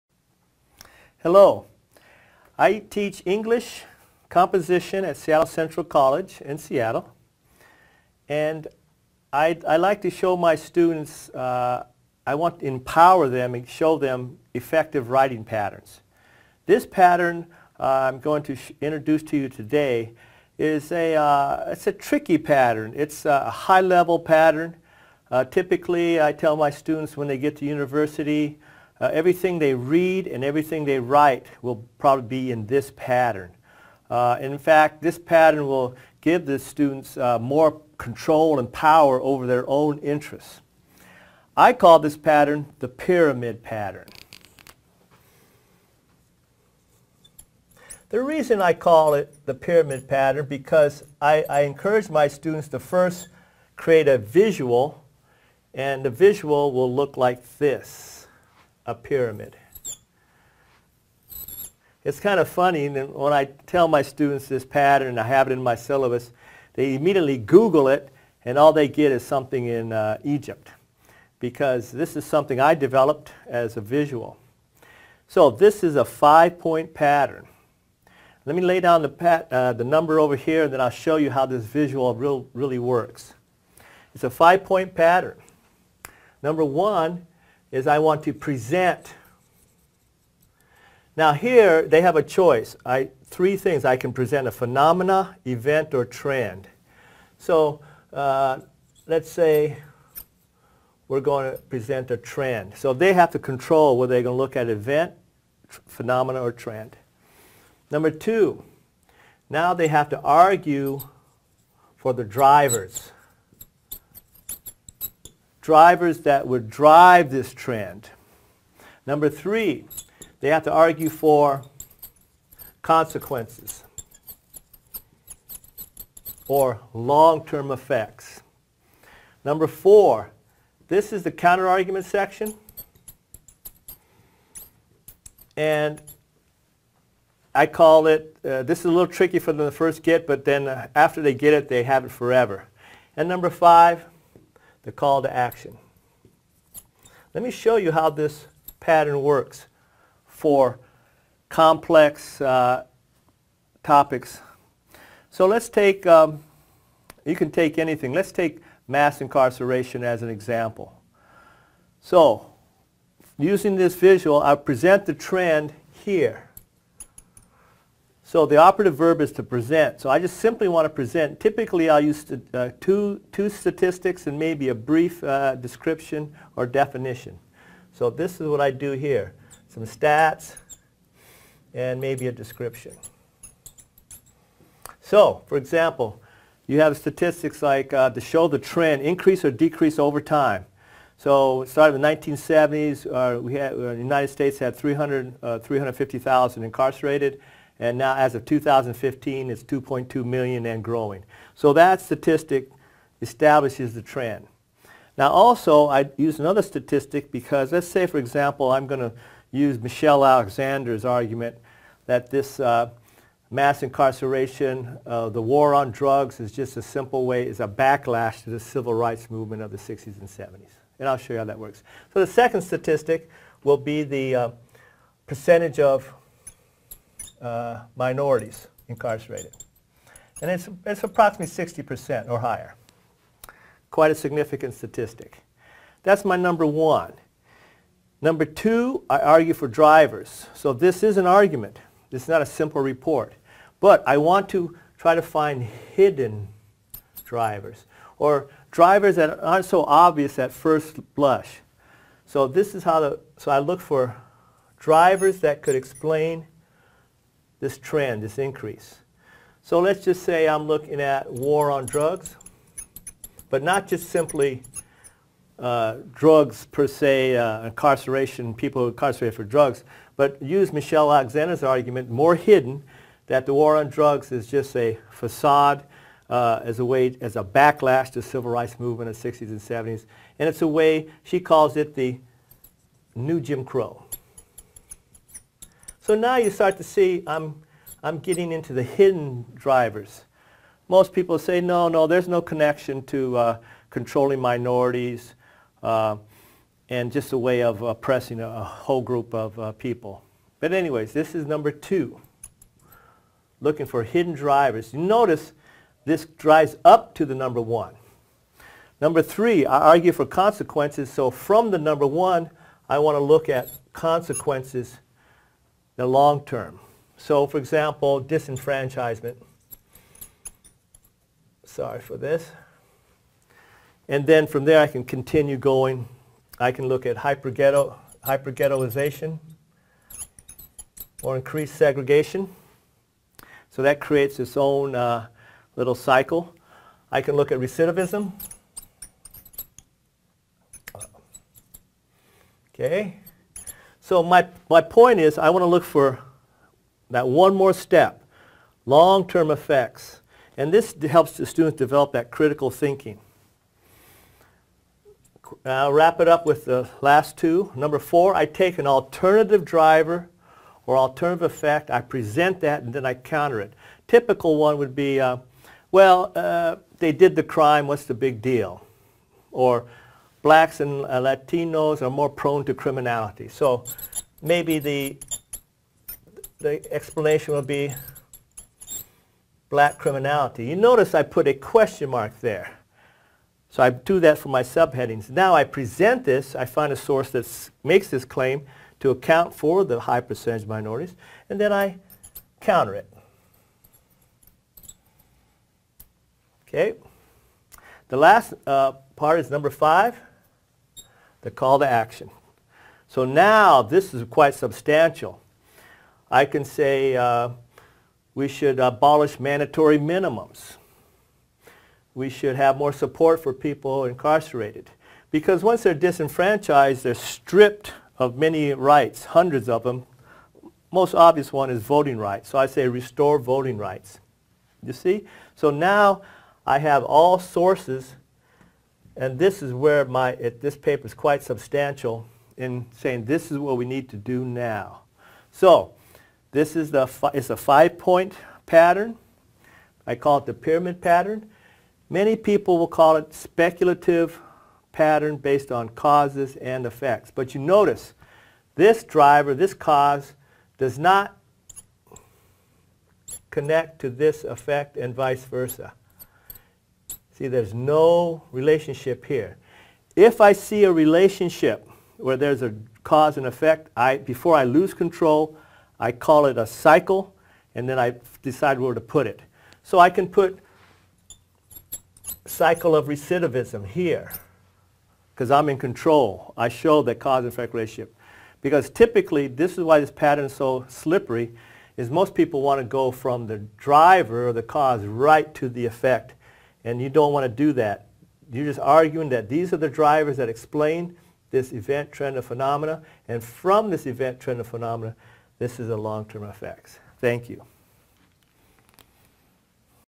Introduction to the Five Point Pyramid (speculation on drivers and consequences) Writing Pattern
Speculating on Drivers and Consequences
By Professor Paul Croon of Seattle Central College
Recorded on a lightboard.